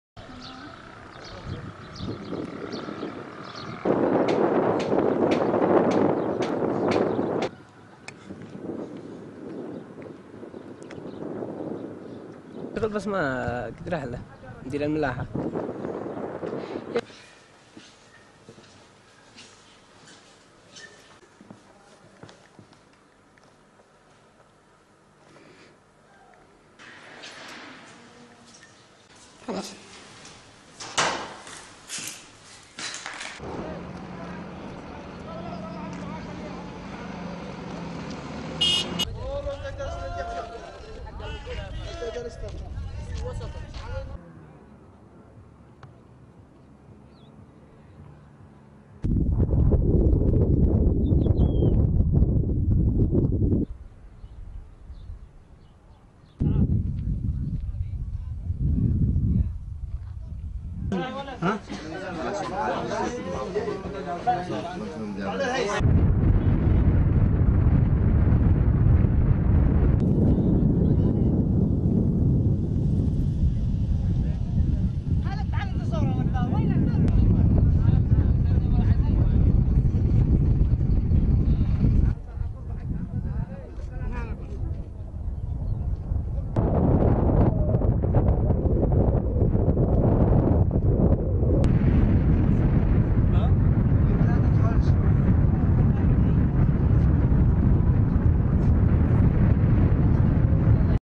Saudi led warplanes bomb Sanaa Airport in Yemen
The video displays damaged airport building and planes after Saudi-led coalition warplanes staged airstrikes at Sanaa International Airport (SAH) and Al-Dulaimi military airbase, located near SAH, on April 29, 2015. "The raids destroyed the runway, along with one plane," Mazen Ghanem of Yemen's aviation authority said.